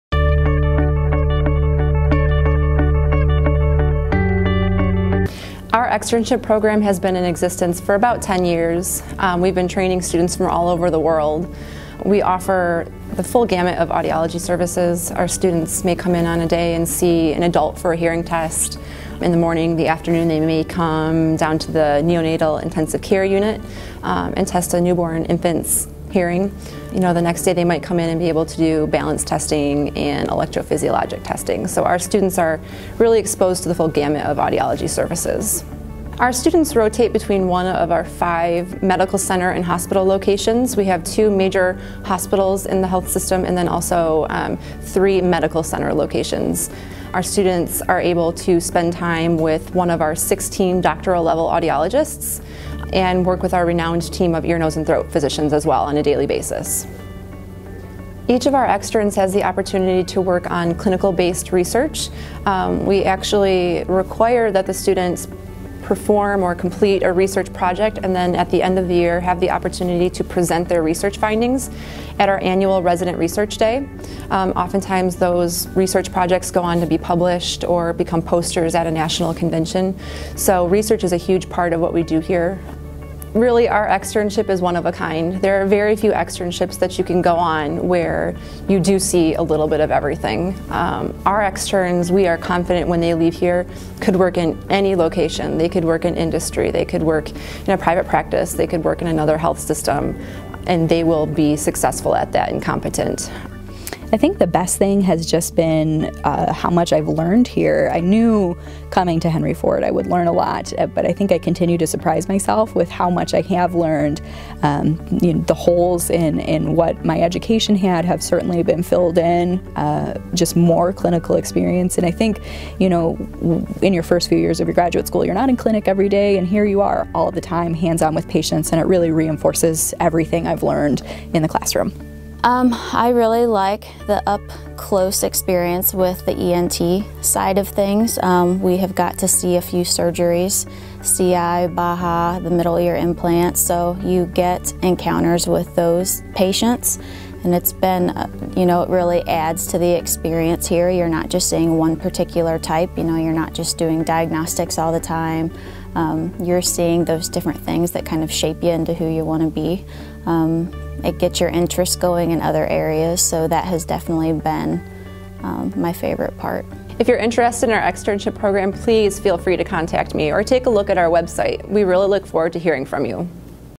Audiology Externship at Henry Ford
A look inside the Audiology Externship program offered at Henry Ford Health System in and around Detroit, MI.